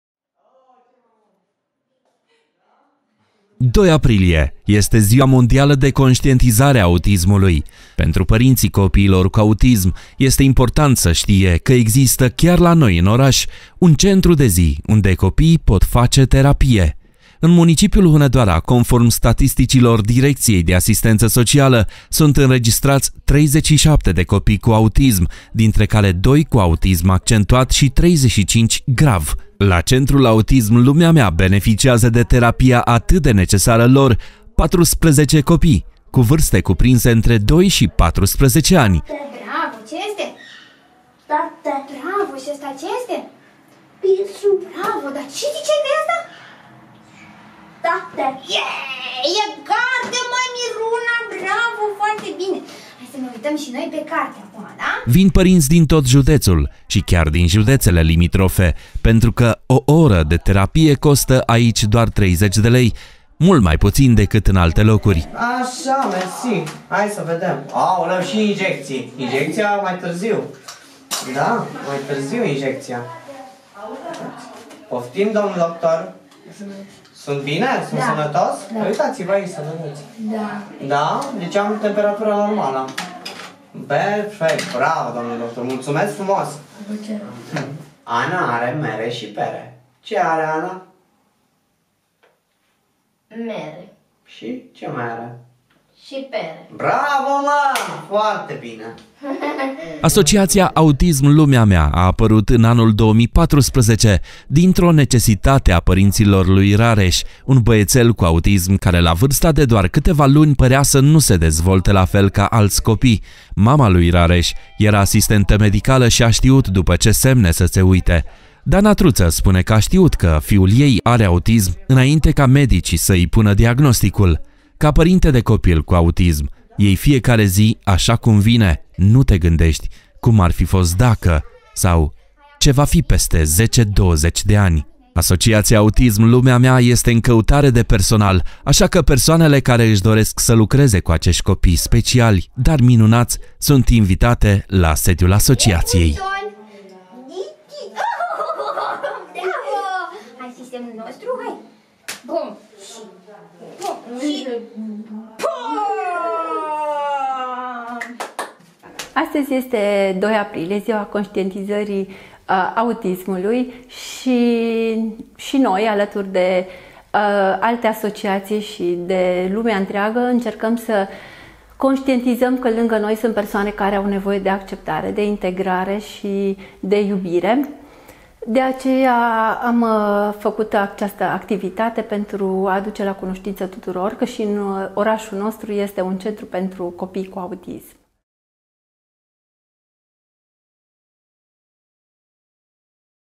2 Aprilie - Ziua Mondială de Conștientizare a Autismului La Asoc. Autism „Lumea Mea” din Hunedoara
2 aprilie este Ziua Mondială de Conștientizare a Autismului. Pentru părinții copiilor cu autism, este important să știe că există chiar la noi în oraș un centru de zi unde copiii pot face terapie.

În municipiul Hunedoara, conform statisticilor Direcției de Asistență Socială, sunt înregistrați 37 de copii cu autism, dintre care 2 cu autism accentuat și 35 – grav.

La centrul Autism – Lumea mea beneficiază de terapia atât de necesară lor 14 copii cu vârste cuprinse între 2 și 14 ani. Vin părinți din tot județul și chiar și din județele limitrofe, pentru că o oră de terapie costă aici 30 de lei, mult mai puțin decât în alte locuri.

Asociația Autism – Lumea mea a apărut în anul 2014, dintr-o necesitate a părinților lui Rareș, un băiețel cu autism care, la vârsta de câteva luni, părea să nu se dezvolte la fel ca alți copii. Mama lui Rareș era asistentă medicală și a știut după ce semne să se uite. Dana Truță spune că a știut că fiul ei are autism înainte ca medicii să îi pună diagnosticul.

Ca părinte de copil cu autism, iei fiecare zi așa cum vine, nu te gândești: „cum ar fi fost dacă” sau „ce va fi peste 10-20 de ani”.

Asociația Autism – Lumea mea este în căutare de personal, așa că persoanele care își doresc să lucreze cu acești copii speciali sunt invitate la sediul asociației.